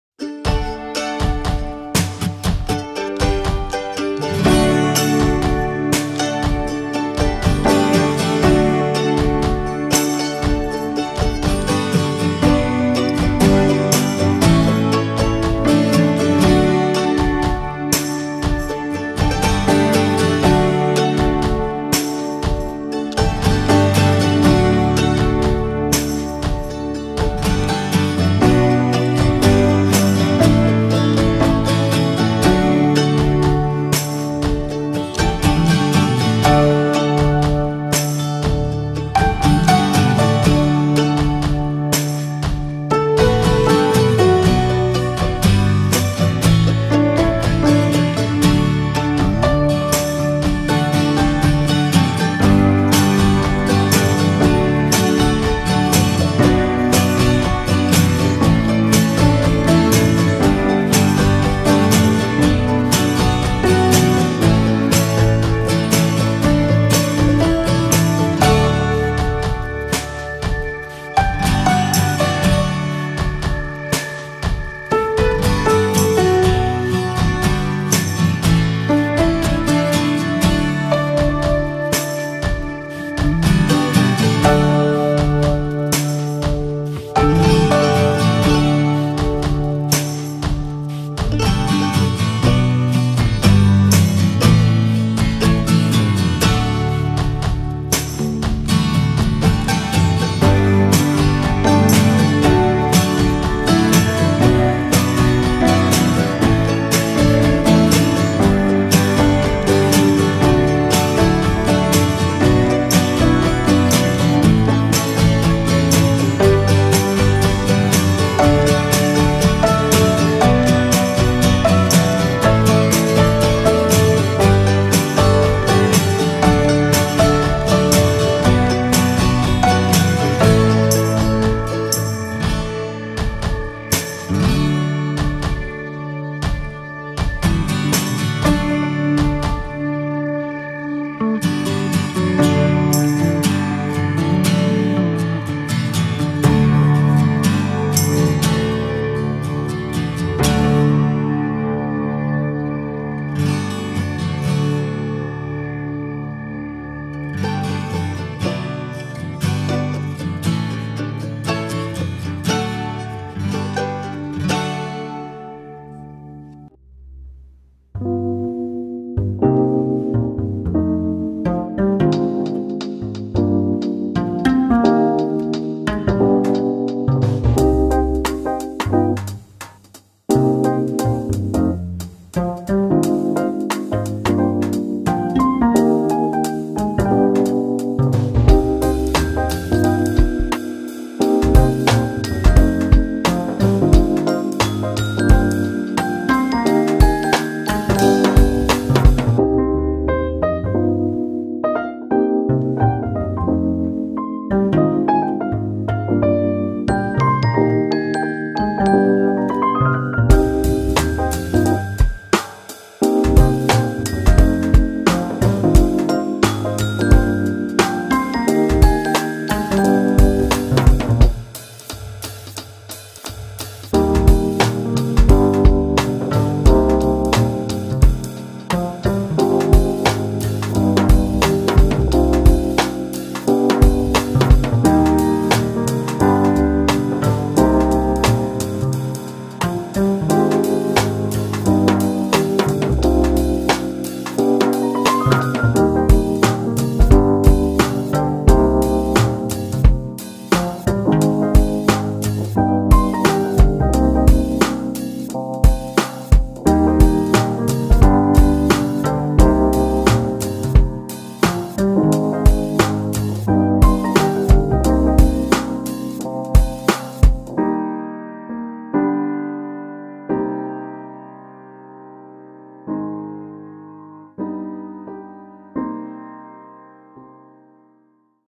How to make Salami with eggs and sweet Onion....المرتديلا اللذيذة مع البيض والبصل الفريك
A delicious meal the way to prepare it is simple ,and within ten minutes, it is ready to serve and its ingredients are available .please enjoy a pleasant meal.
وجبة لذيذة تستطيع تحضيرها في عشر دقائق
مكوناتها بسيطة ومتوفرة